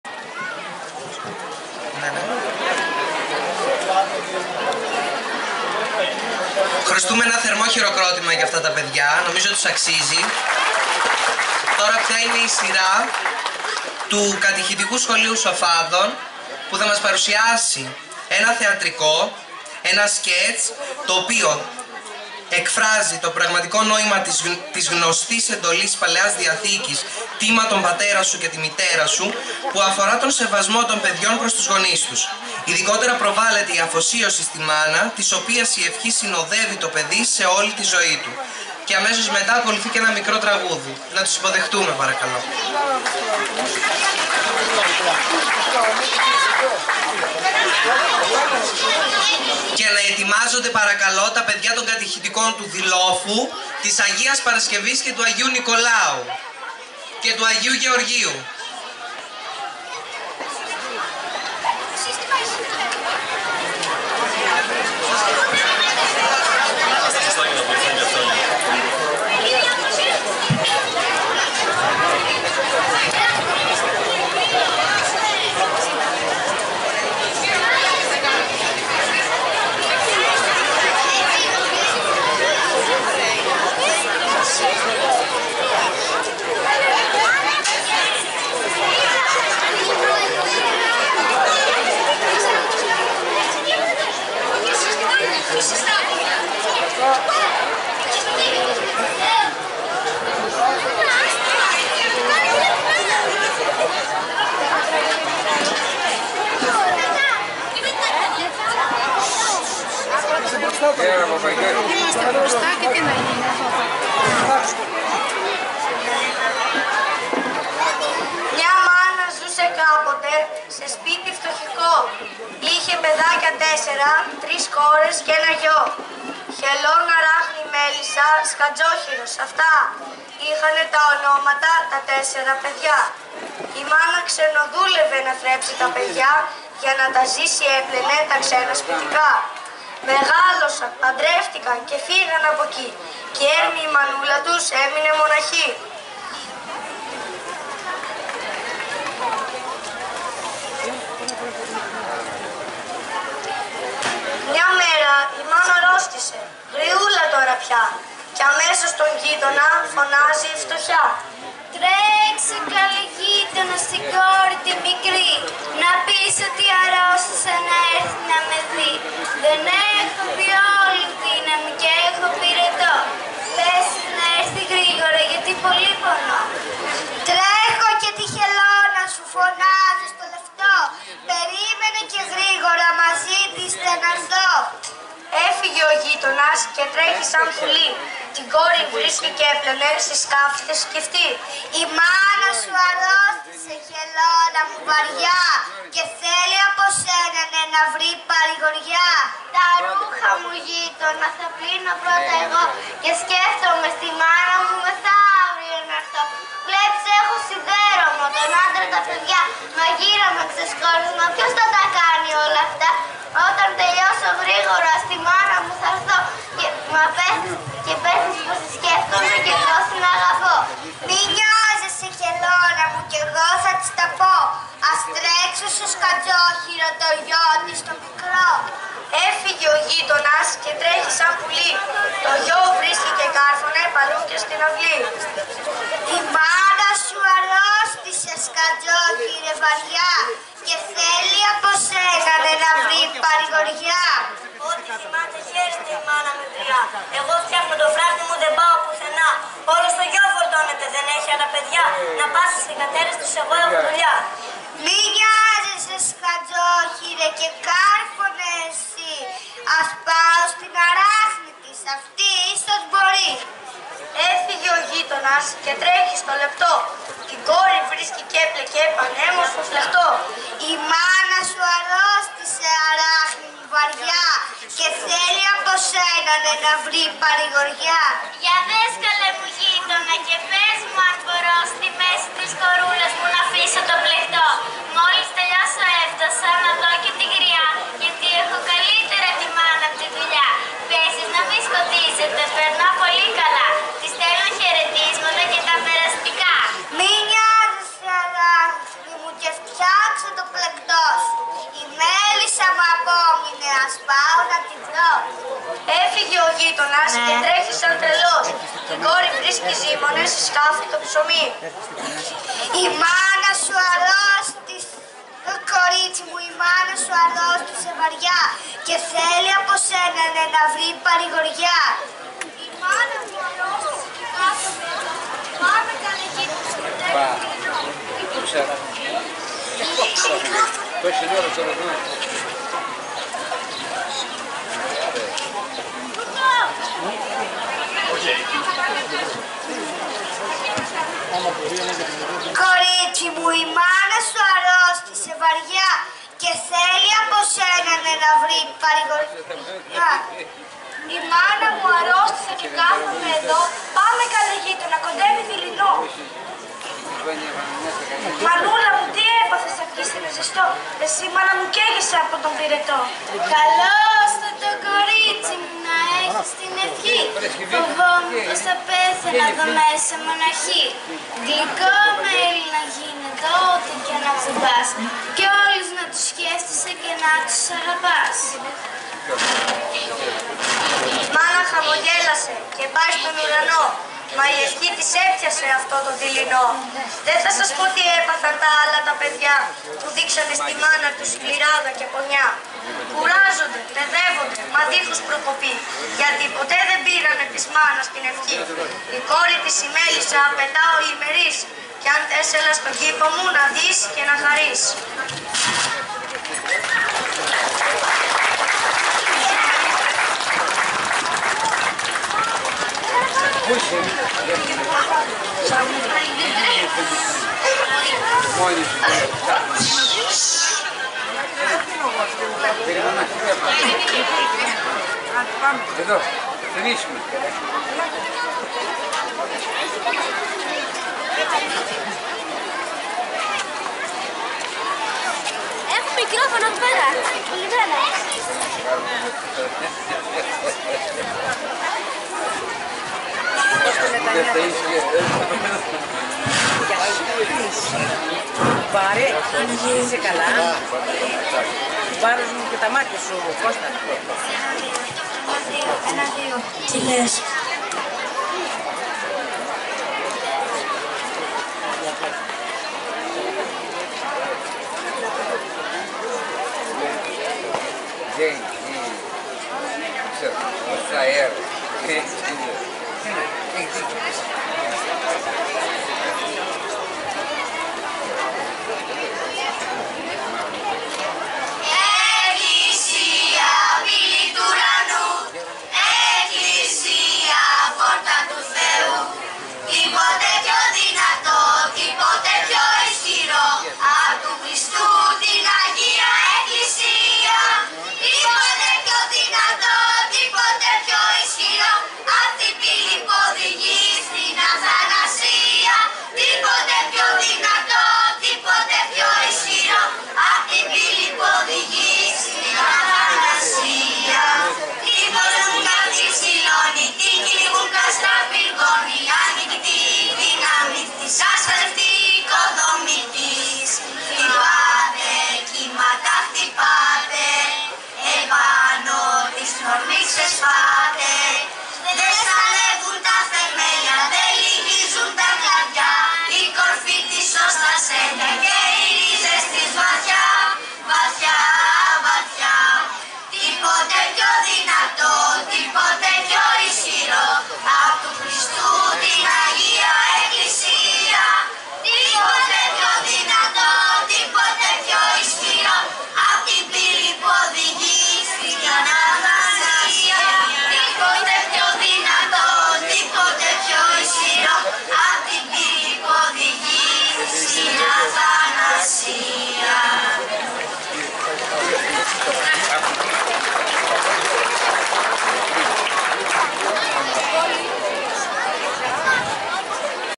ΚΑΤΗΧΗΤΙΚΟ ΣΧΟΛΕΙΟ ΣΟΦΑΔΩΝ
ΤΟ ΣΑΒΒΑΤΟ 6 ΜΑΙΟΥ 2017  ΠΡΑΓΜΑΤΟΠΟΙΗΘΗΚΕ Η ΤΕΛΕΤΗ ΛΗΞΗΣ ΤΩΝ ΚΑΤΗΧΗΤΙΚΩΝ  ΣΥΝΑΞΕΩΝ ΤΗΣ ΙΕΡΑΣ ΜΑΣ ΜΗΤΡΟΠΟΛΕΩΣ ΣΤΟ ΠΟΛΙΤΙΣΤΙΚΟ ΚΕΝΤΡΟ ΦΑΡΣΑΛΩΝ